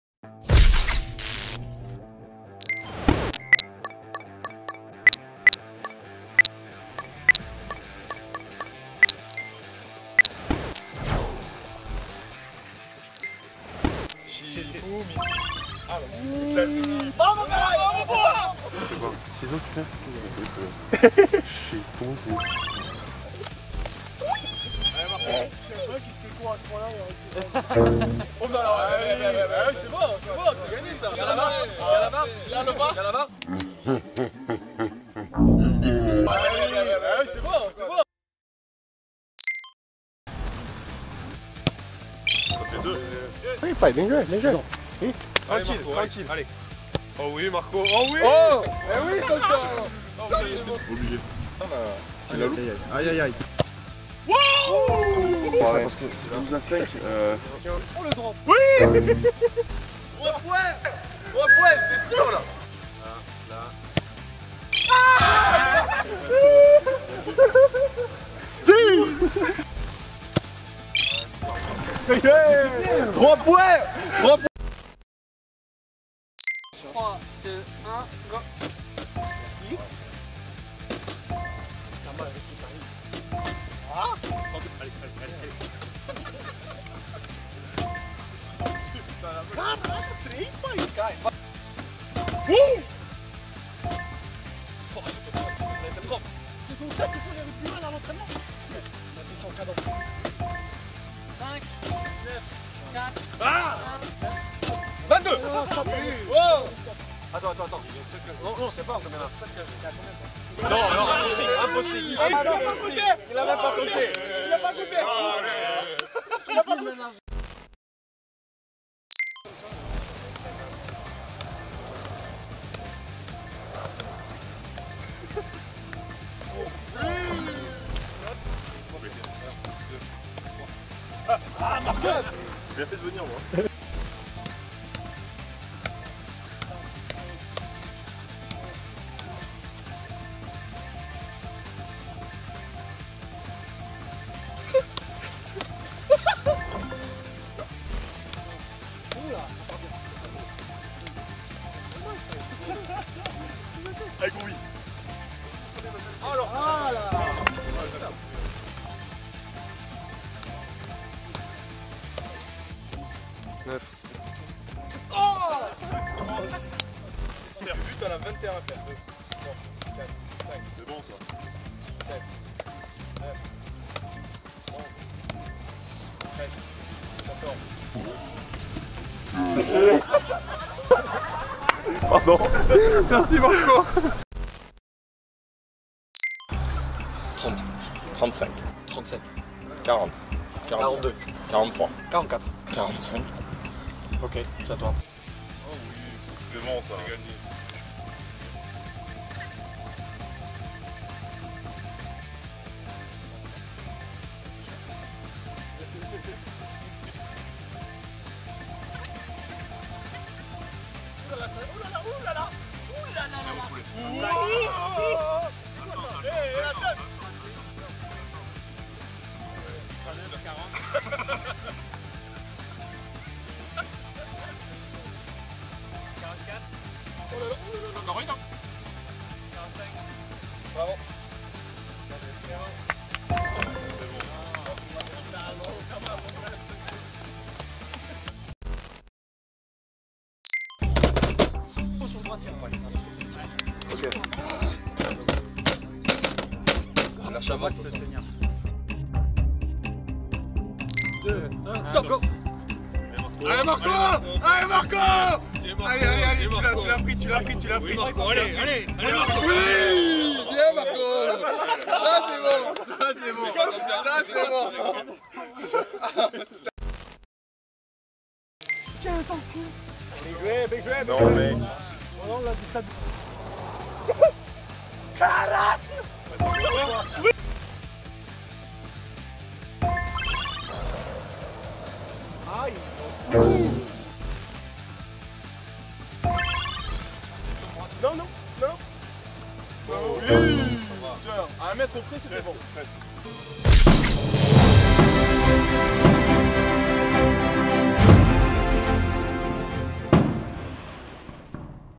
Betclic Challenge UBB vs Girondins de Bordeaux
💪 Qui remportera ce tout premier BetclicChallenge bordelais ?
🏉 Le casting rugby : Marco Tauleigne, Lucas Meret et Romain Buros
⚽️ Le casting football : Yacine Adli, Pablo, Loris Benito
🎯 Les disciplines : chifoumi, adresse pieds et main, bottle flip challenge, pompes et bras de fer !
(tournage réalisé début 2020 avant la période de confinement)